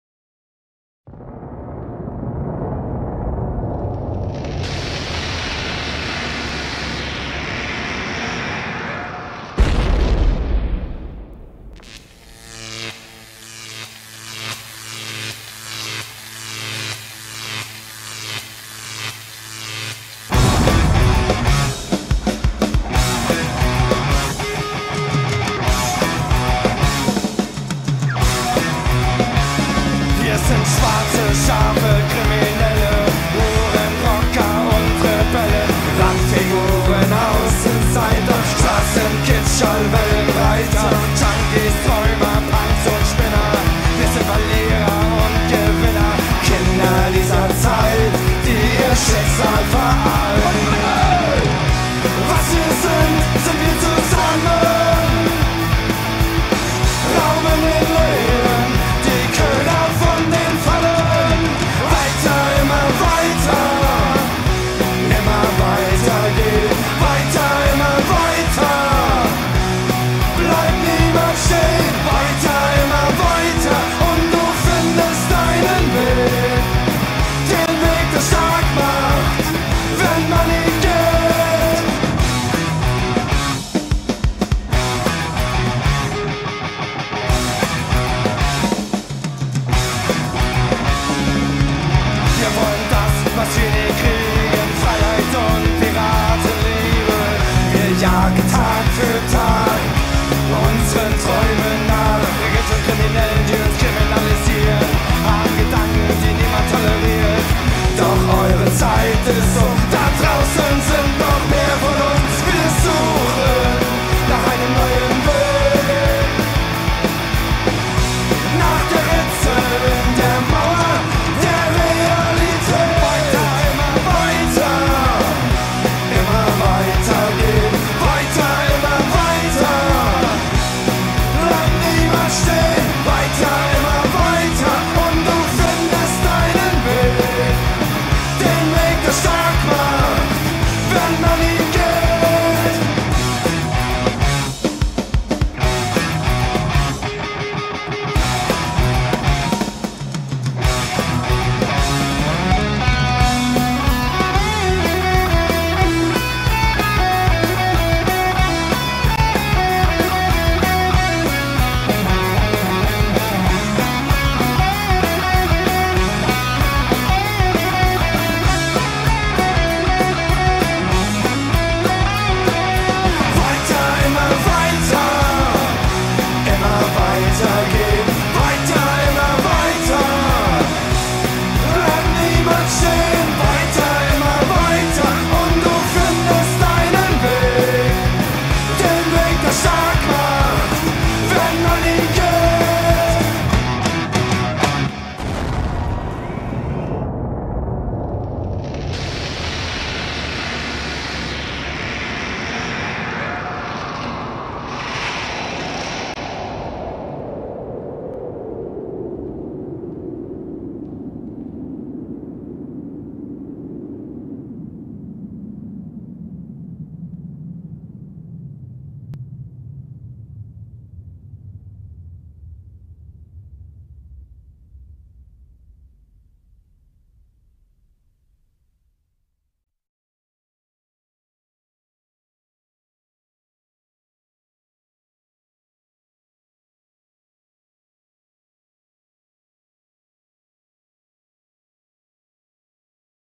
6 Jahre Glorreiche Halunken 11/12/13.06.2010 in 06682 Teuchern
Das Jubiläumsfestival in 06682 Teuchern, Eintritt Frei:))) + Fussball WM auf Großbildleinwand